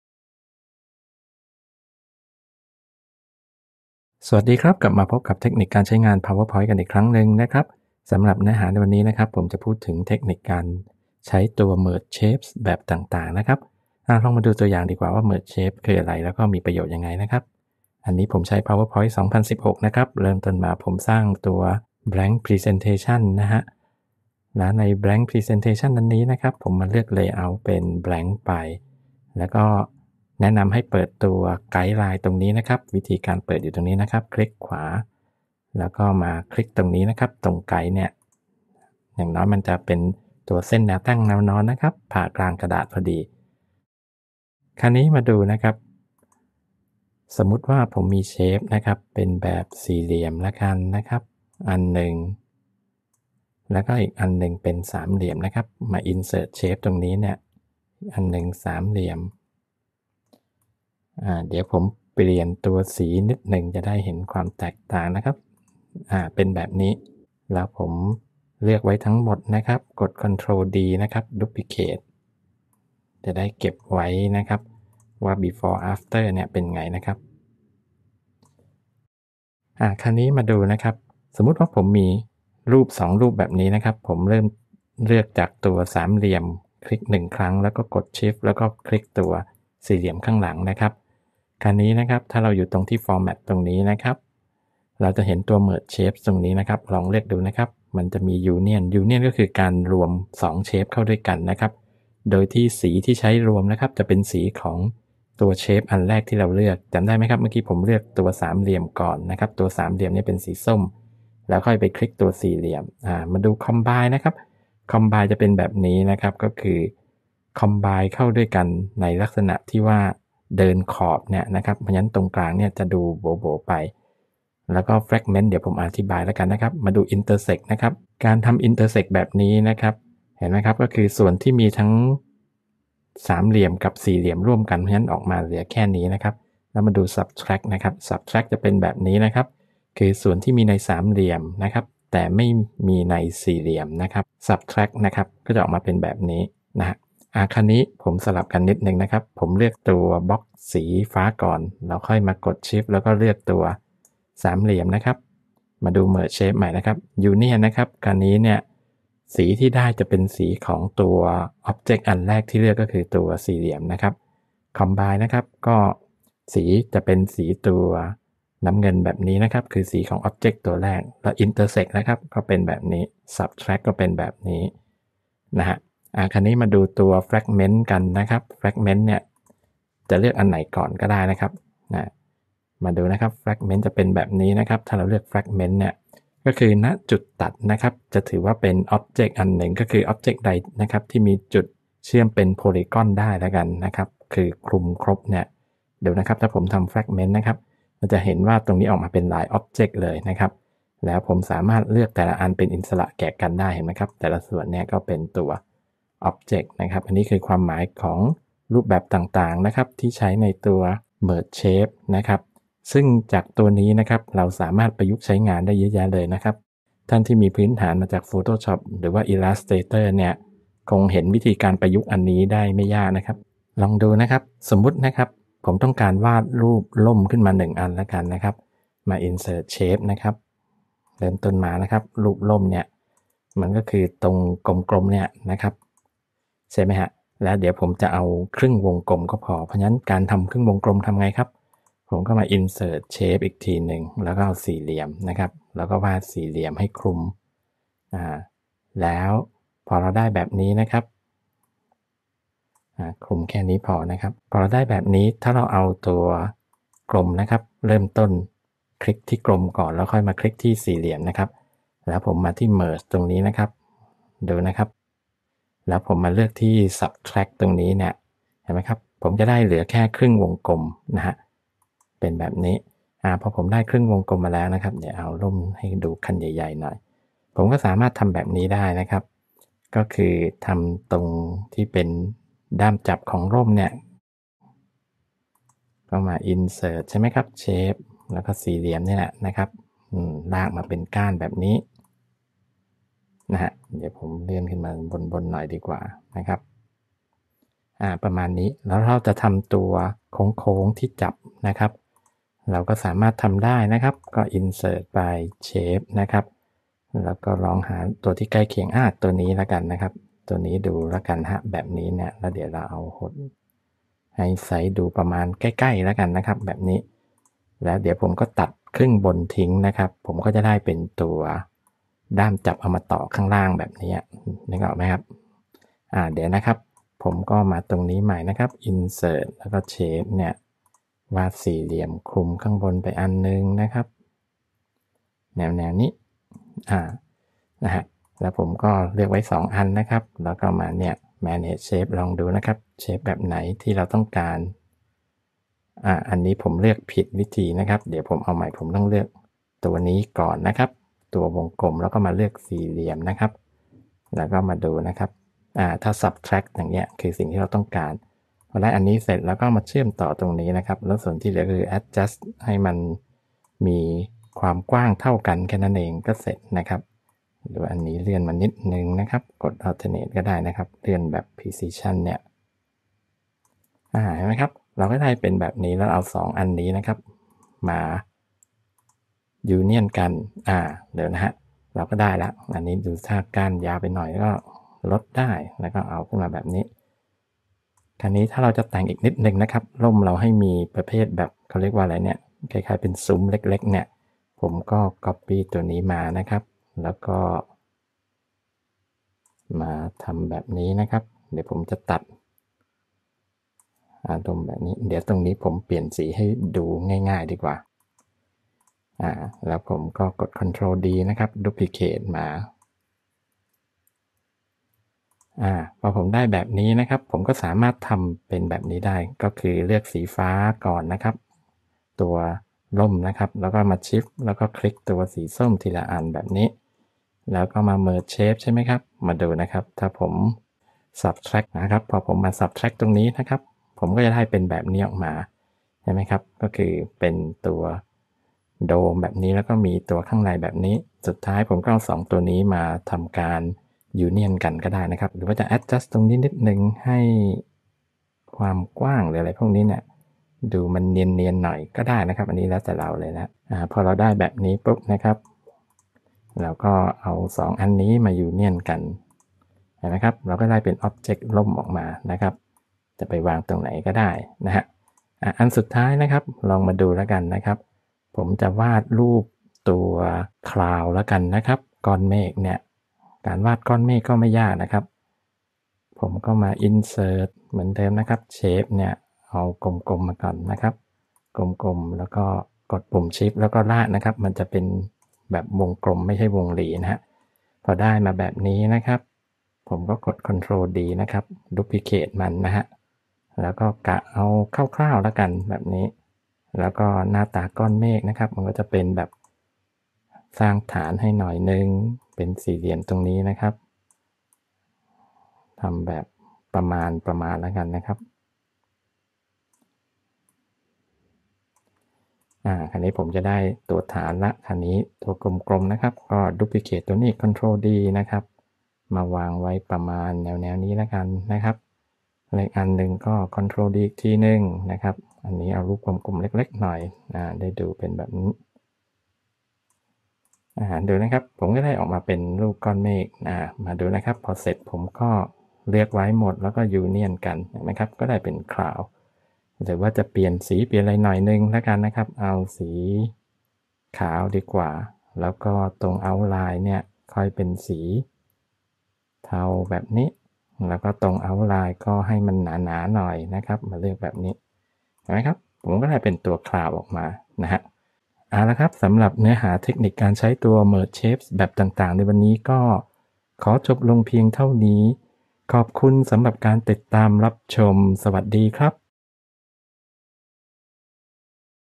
สอน PowerPoint: การประยุกต์ใช้ Merge shapes แบบต่าง ๆ
เทคนิคการใช้ merge shapes (union, combine, fragment, intersect, subtract) ในการสร้างรูปทรง (shape) ใหม่ขึ้นมาจากการผสมผสานรูปทรงอื่น ๆ เช่น การสร้าง ร่ม ก้อนเมฆ (จากรูปทรงกลม และสี่เหลี่ยม)
playlist สอนเทคนิคการสร้างกราฟ แผนภูมิแบบต่าง ๆ ด้วย Excel